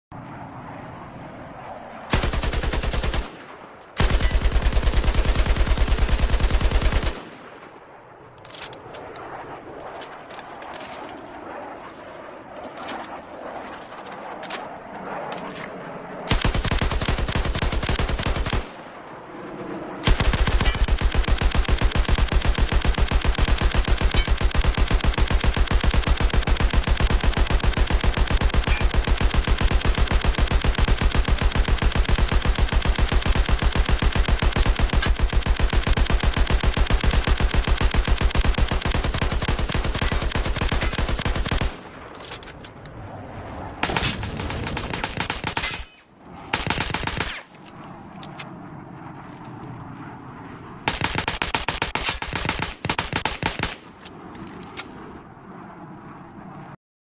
CronusMAX - Anti Recoil and Primary/Secondary RapidFire
Showing off the Anti Recoil and Primary/ Secondary RapidFire features. More accurate aiming when you need it and faster shooting when you're in panic mode